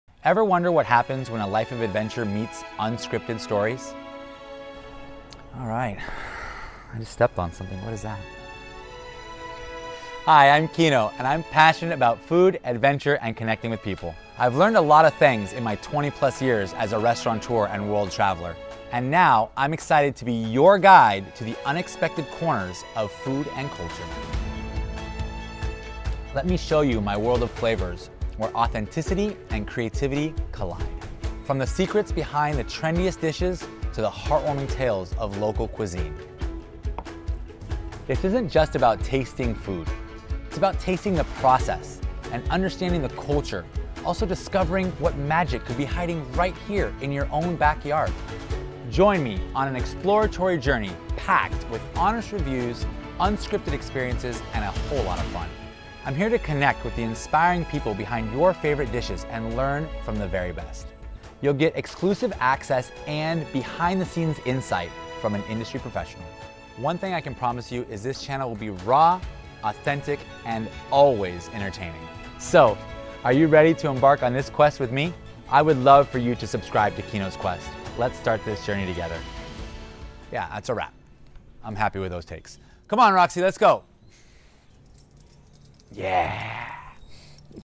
Keeno's Quest Trailer
Hungry for adventure? Consider me your go-to culinary explorer! I've been passionate about food, thrills, and making new connections for over twenty years during my wild adventures and world travels—and I'm here to share it all.

New episodes uploaded bi-weekly on Tuesdays!